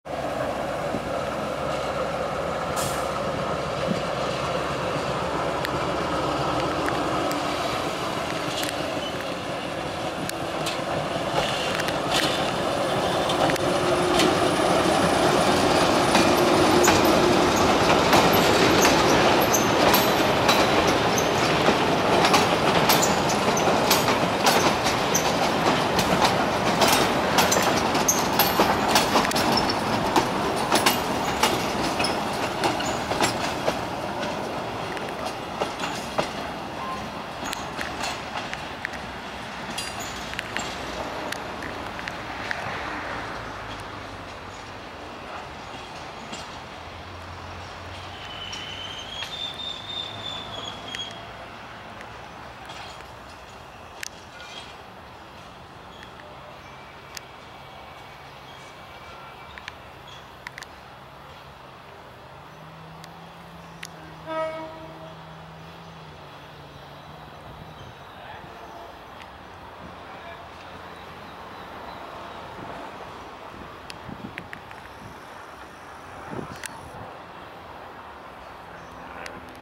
U-10 101 con rielero de LBS (14-02-2018)
Trenes Argentinos - Línea Belgrano Sur - Siendo las 10:45 hs, me encontré con ésta sorpresa en Tapiales, la locomotora General Electric U-10 101, saliendo de un desvío que se utiliza como depósito hacía playa Tapiales, con un tren rielero conformado por dos chatas cargadas y el furgón de cola 394544. Miércoles 14 de Febrero de 2018.